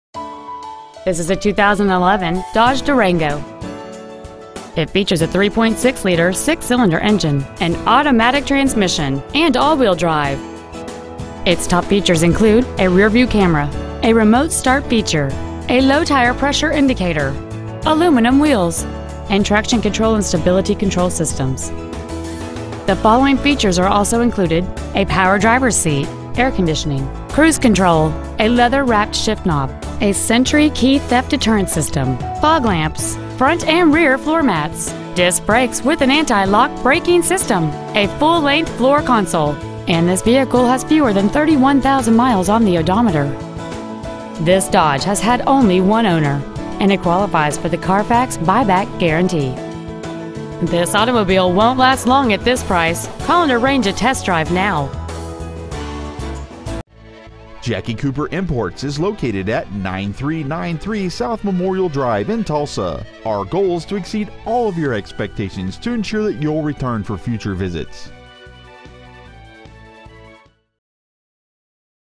2011 Dodge Durango Tulsa OK 74133
Year : 2011
 Make : Dodge
 Model : Durango
 Engine : 3.6L 24-VALVE V6 VVT ENGINE
 Trans . : Automatic
 Exterior : Gray
 Miles : 30,482

 Stock : BC743098